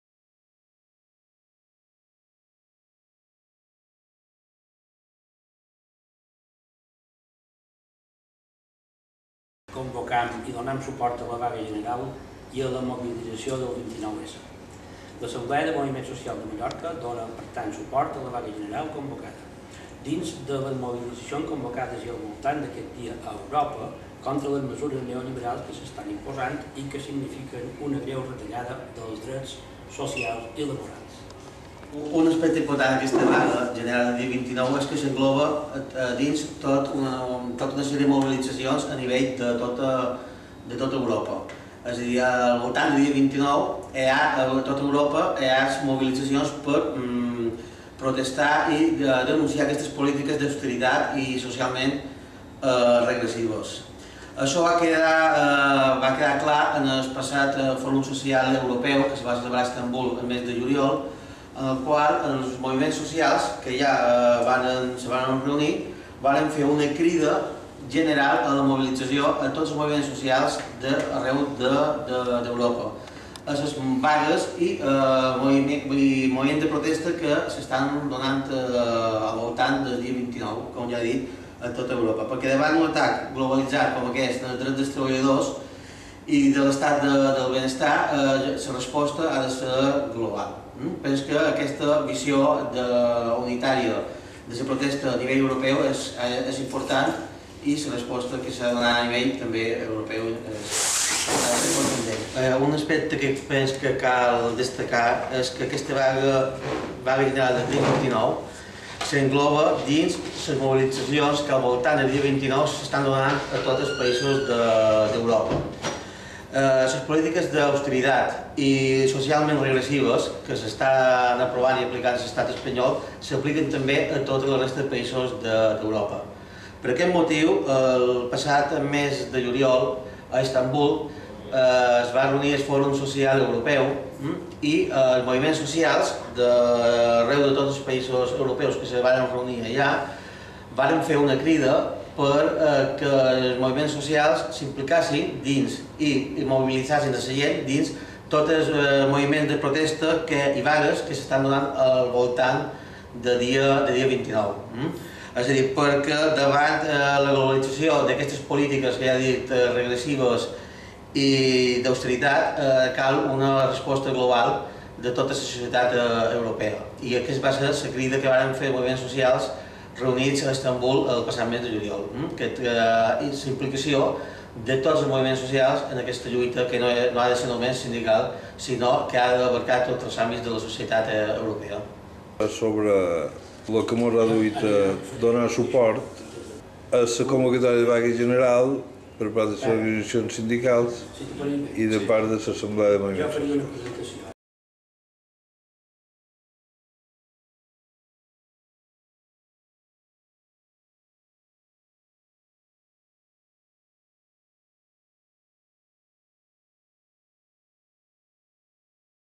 Assemblea de Moviments Socials de Mallorca dóna suport a VAGA GENERAL (4)
Imatges de la intervenció del representant de l'STEI-i a l'Assemblea de Moviments Socials de Mallorca, durant la roda de premsa efectuada en suport a la VAGA GENERAL del 29 de setembre de 2010